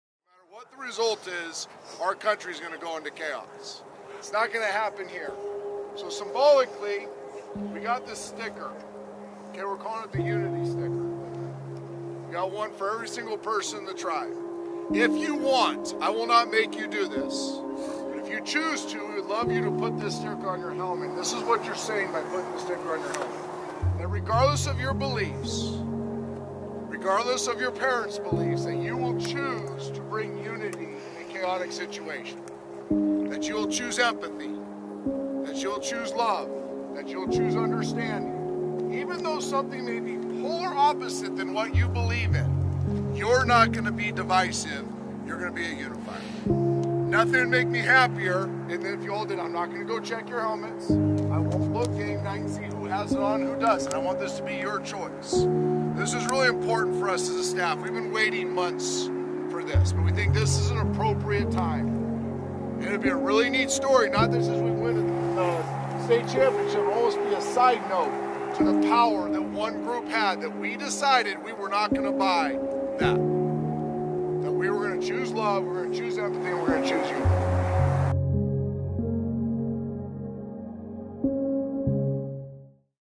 Mustang Unity Stickers
A few weeks ago, we handed out Unity stickers to our tribe to demonstrate the unity and togetherness that we embrace as a team. We are extremely thankful for the way our tribe has surrounded each other no matter our backgrounds or differences